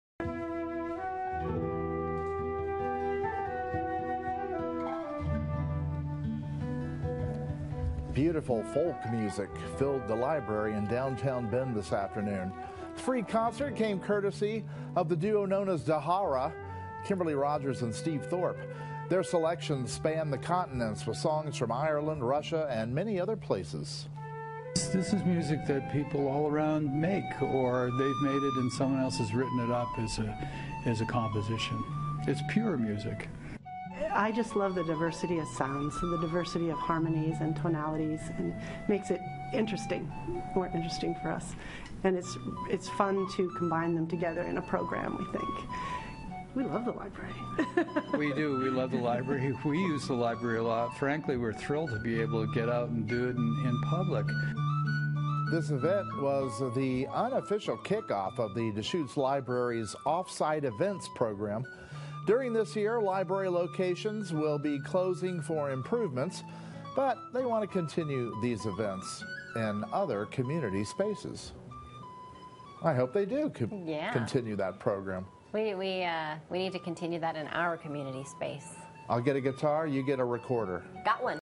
Folk music fills Bend Library in free concert
Folk music from around the world filled the air at the downtown Bend Library Thursday afternoon in a free concert by Steve Thorp and Kimberly Rogers, who make up a duo called Da Chara.